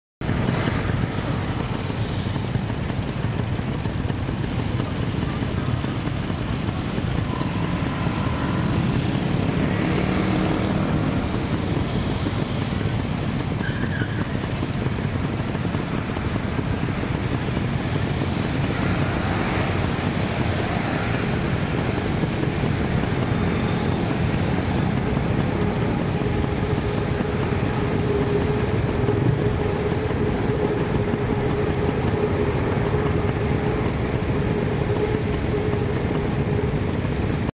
ทดสอบอัพวิดีโอ 4K Huawei Mate 9 ลง Youtube
หลังจากก่อนหน้านี้ Huawei Mate 9 มีปัญหาในการอัพโหลดไฟล์วิดีโอความละเอียด 4K ลง Youtube ไม่ได้วันนี้ Huawei ได้อัพเดท เฟิร์มแวร์ เวอร์ชั่นล่าสุด ซึ่งสามารถอัพไฟล์ 4K ได้แล้ว หมดปัญหาแล้วนะ ใครยังไม่อัพเวอร์ชั่น รีบซะ อิอิ